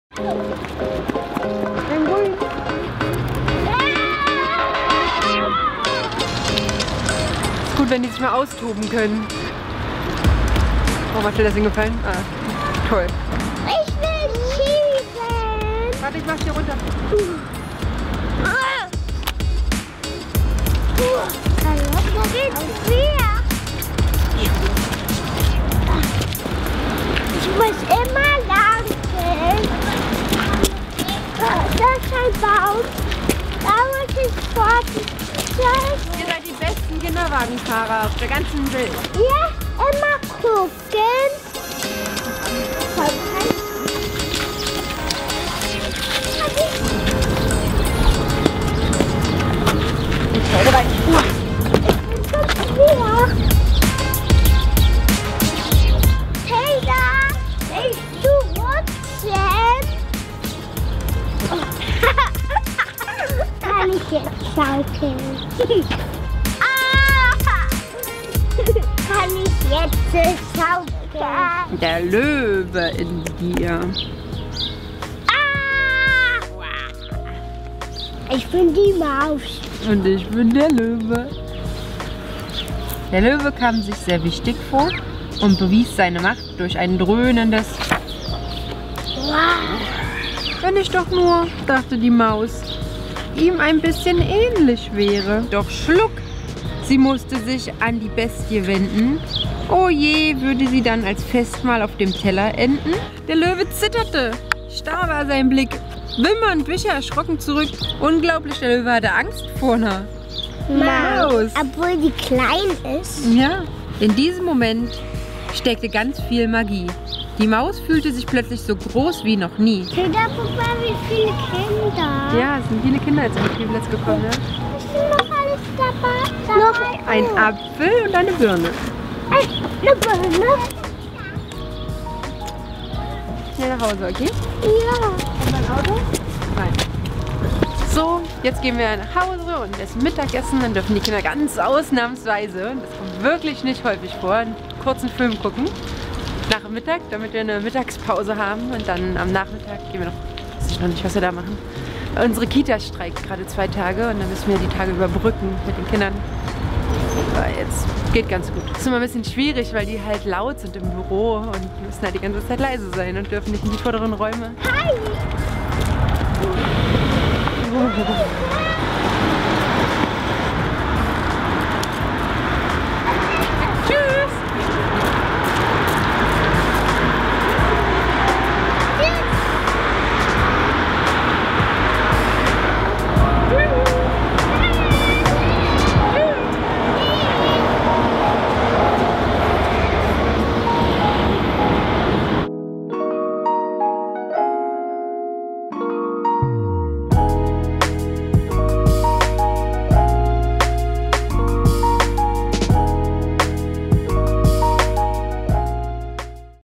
Mama-Alltag mit Sarah - Eine löwenstarke Familie
Heute begleitet ihr Sarah mit den Kindern. Unsere Kita hat zwei Tage lang gestreikt. Also raus, auf den Spielplatz!

zweite Kamera ▸
zweites Stativ ▸
Halterung Auto ▸
Unser Kinderwagen ▸